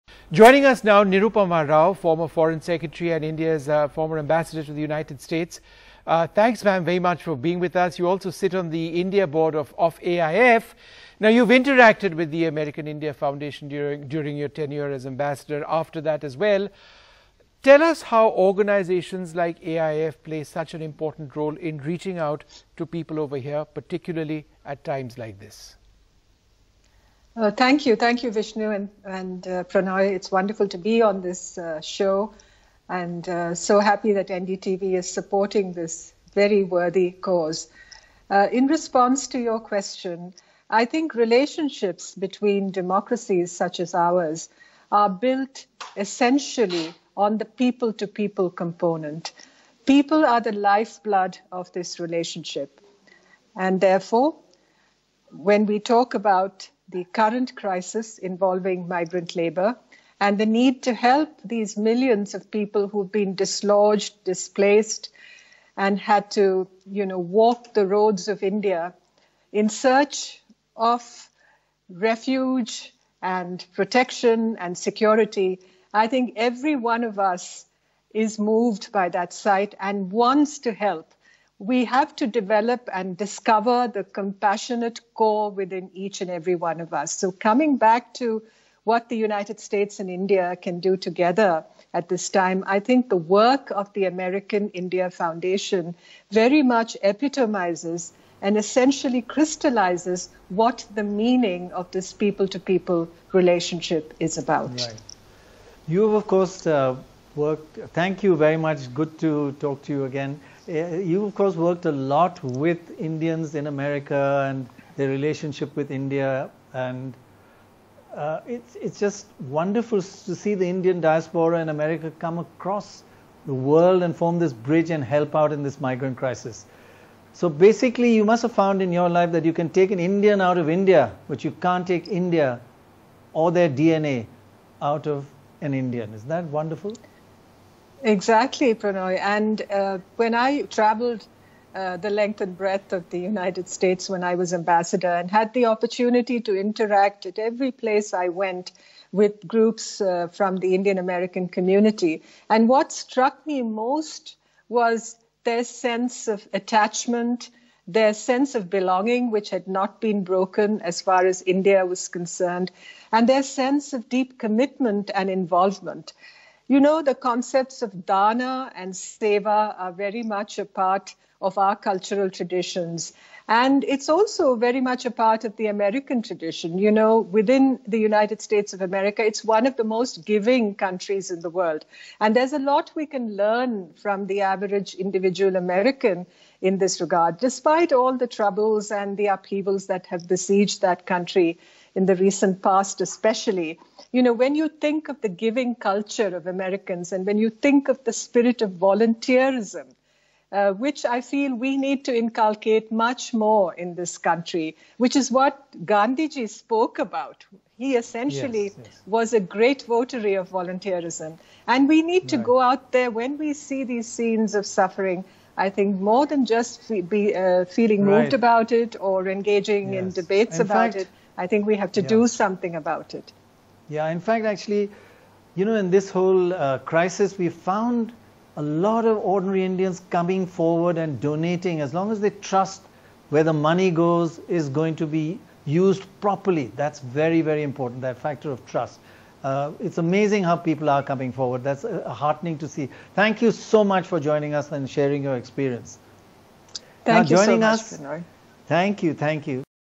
We Need To Help These Millions Of People Who Have Been Displaced: Nirupama Rao
Former Foreign Secretary and India's Ambassador to USA, Nirupama Rao, joined the RebuildingLives telethon where she stressed on the need to help and support the millions of people who have been displaced due to the COVID-19 crisis. She says that these migrant workers are in search of refuge and security and we must come forward to do our bit.